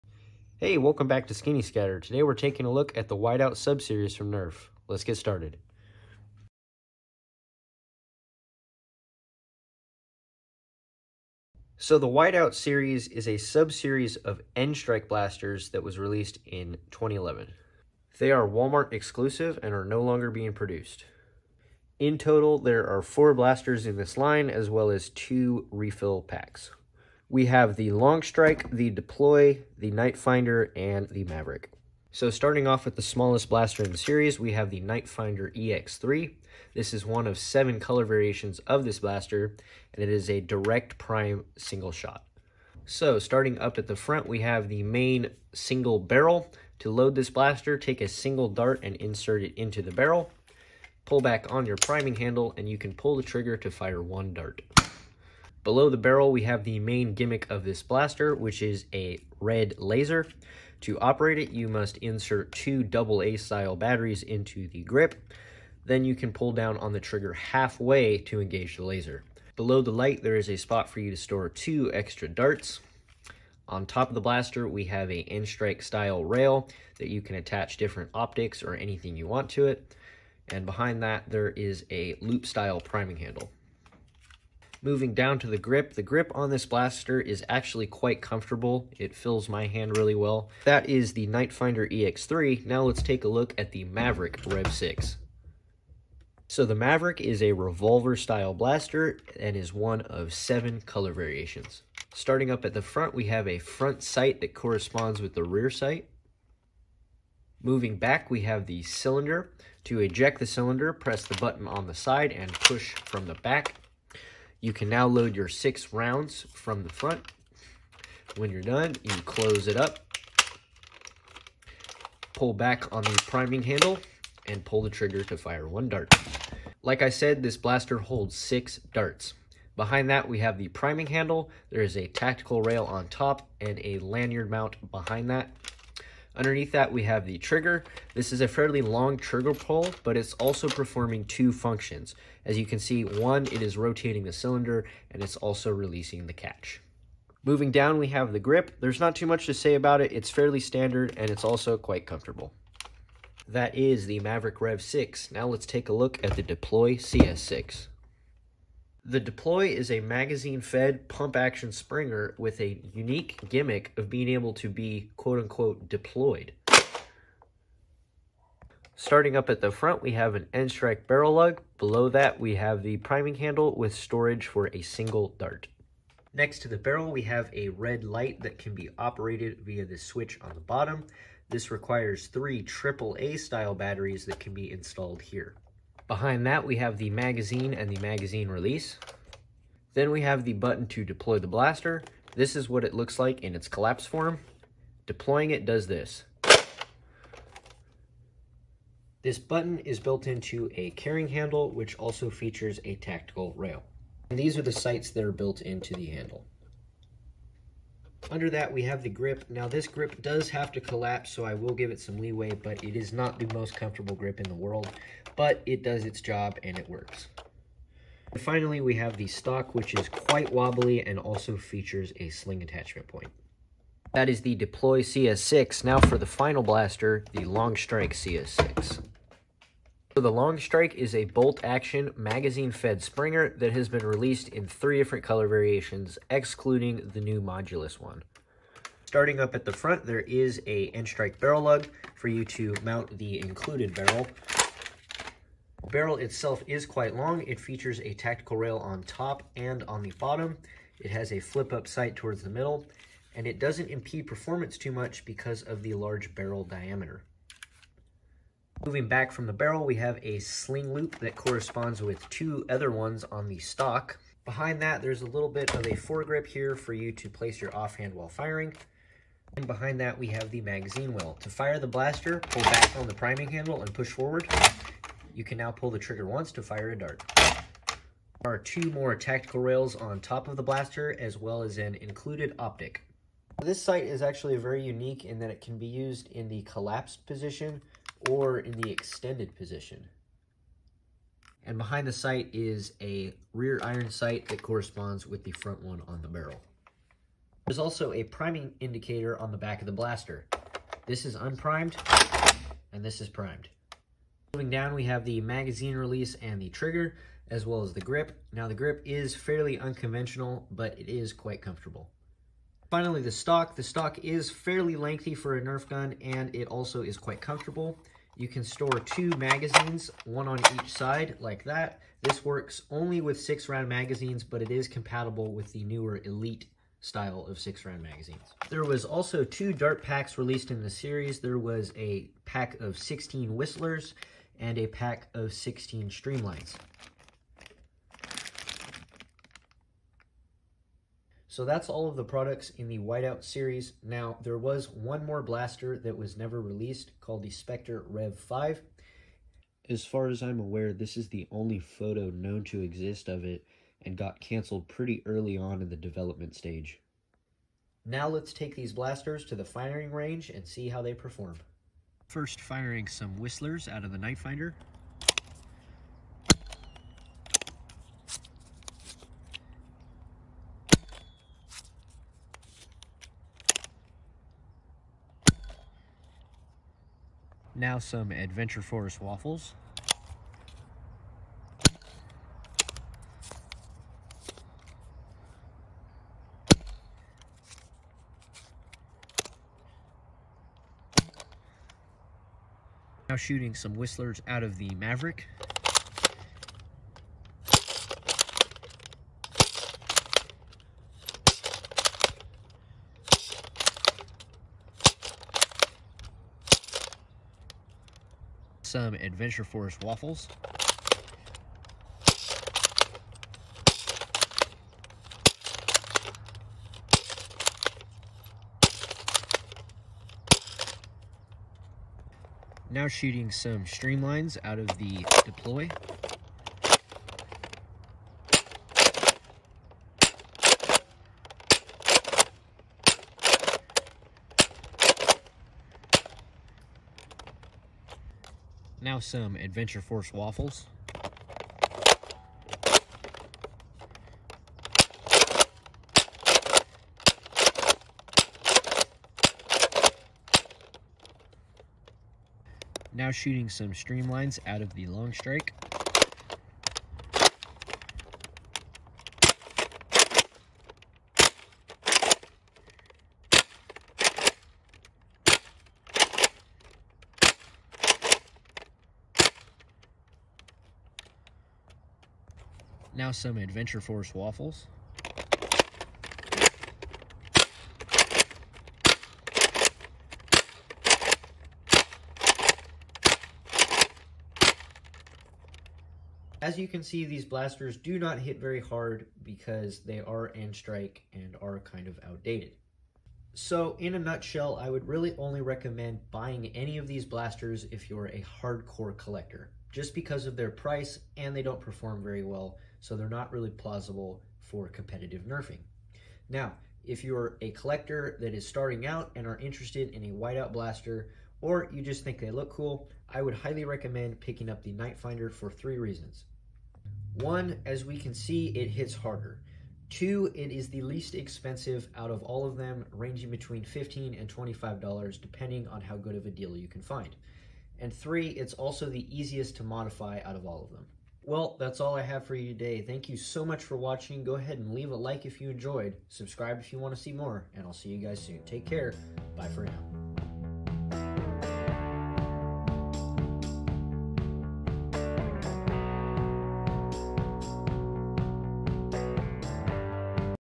[NERF] Series Overview | N-Strike Whiteout Series
Intro: 0:00
Nite Finder: 0:32
Maverick: 1:30
Deploy: 2:33
Longstrike: 3:44
Firing demo: 5:57
Final thoughts: 8:06